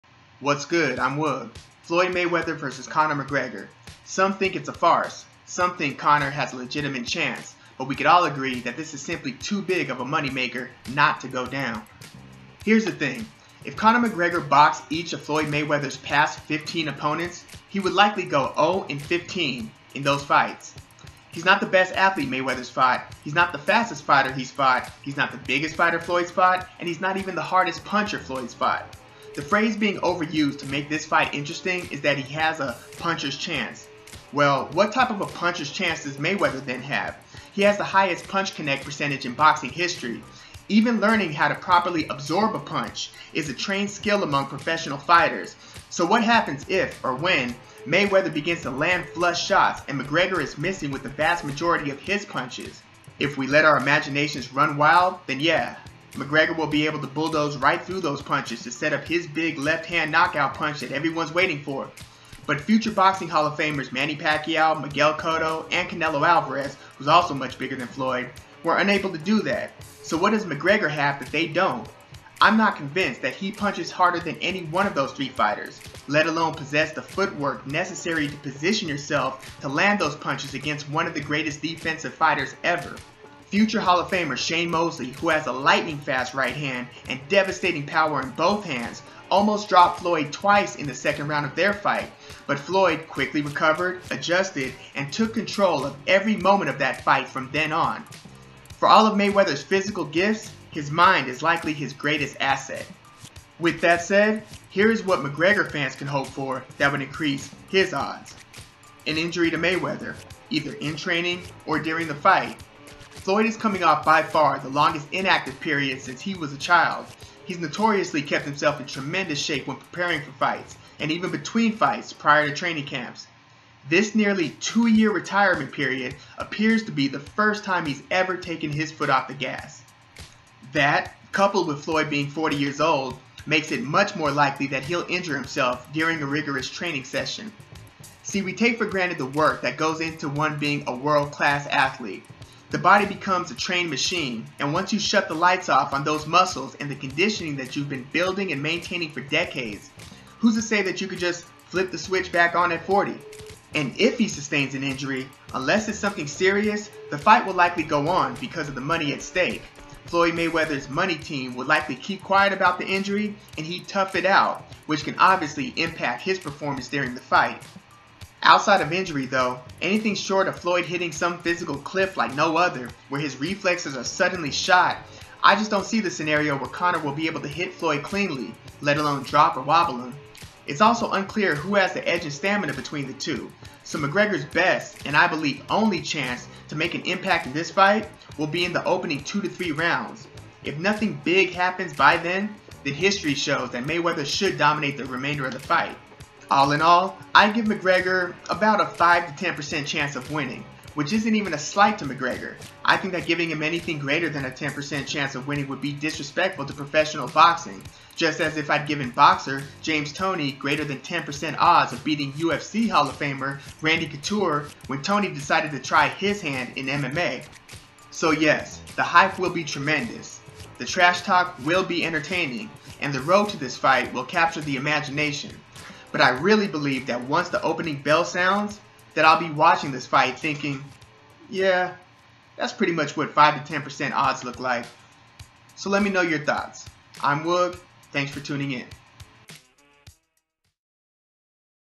Mayweather vs McGregor (Woog's breakdown & prediction)
Woog's take on the superfight between Floyd Mayweather Jr. and Conor McGregor